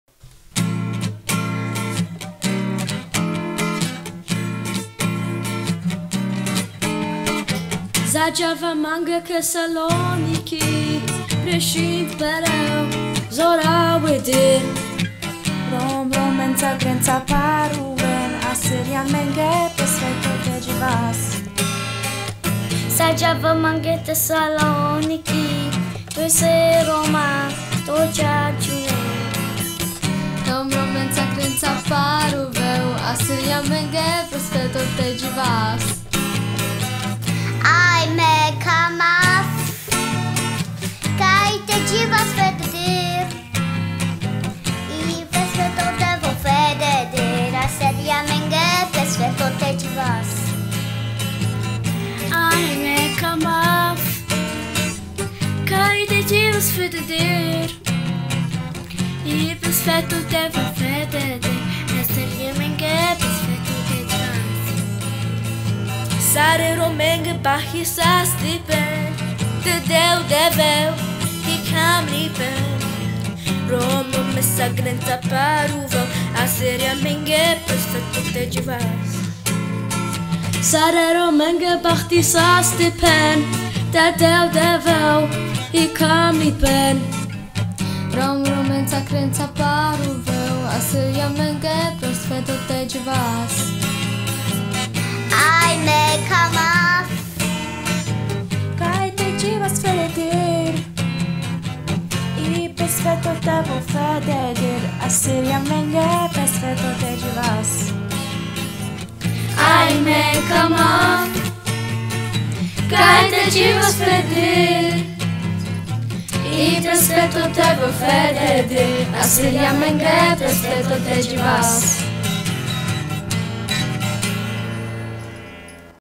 Dzieci i Wnuczki Bogdana Trojanka
Pod fachowym okiem Bogdana Trojanka ruszyły nagrania  dzieci i jego  wnuków.  W przyszłym roku ruszą warsztaty młodzieży w kierunku artystyczno instrumentalno wokalnym